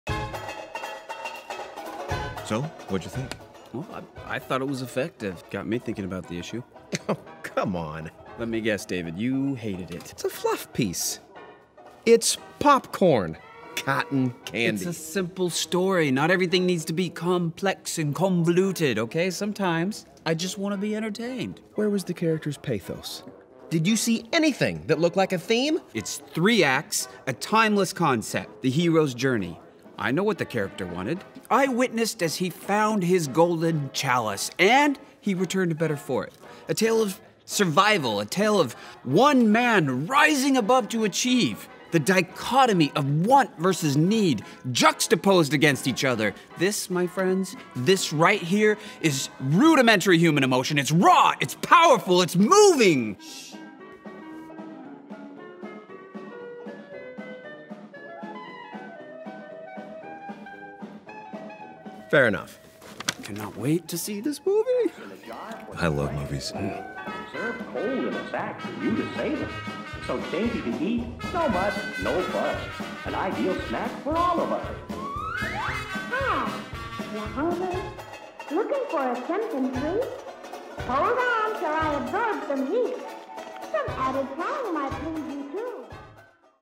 SIFF-RENTON Promo 2015: POPCORN
Adam Gehrke, David Hogan, and Ben Andrews go to the movies!